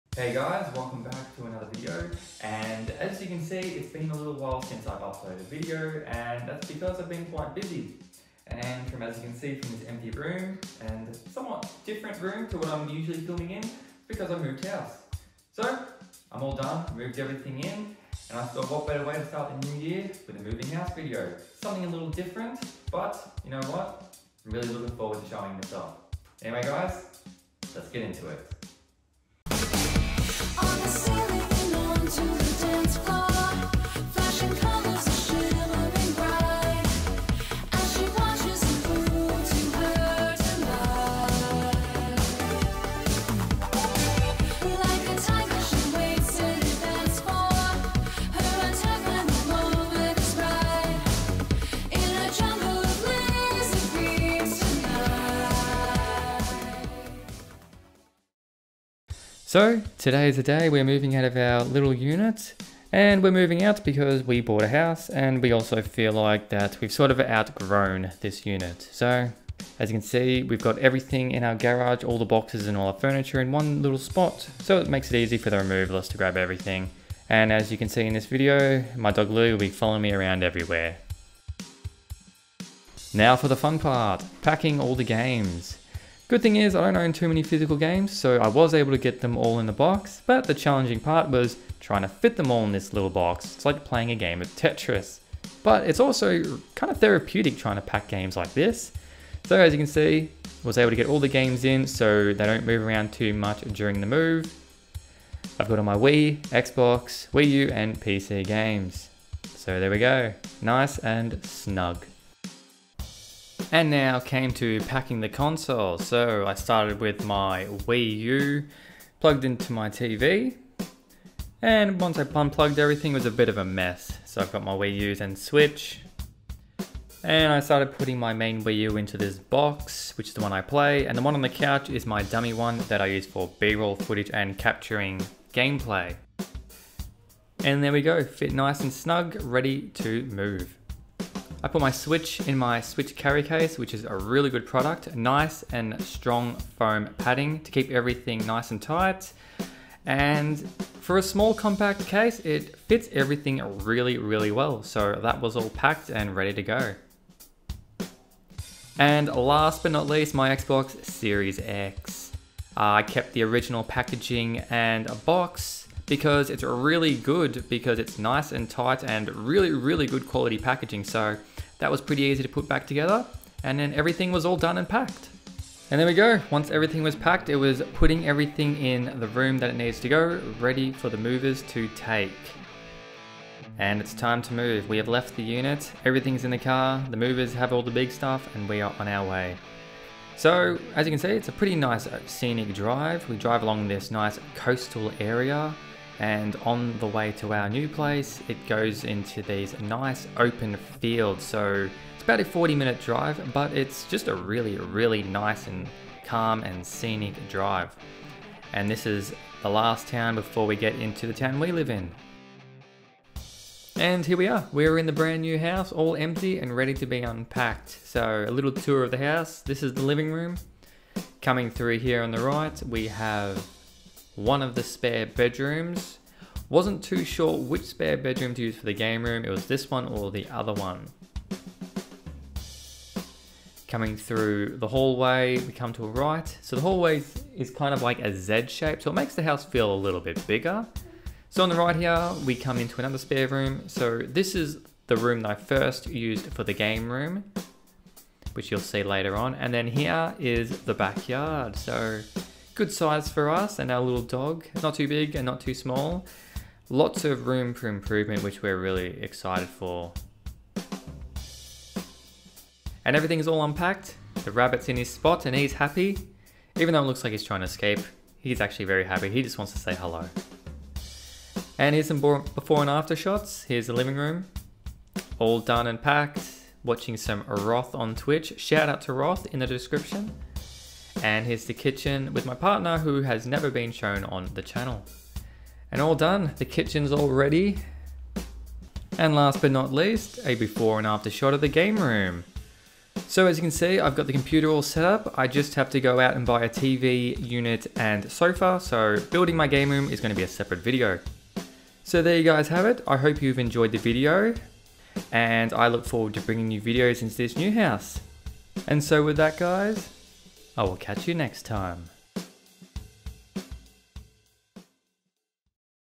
I Am Finally Moving House
Hey guys, today I am finally moving house which is why I have been a little absent from youtube. Hope you guys enjoy this somewhat different video from what I usually upload.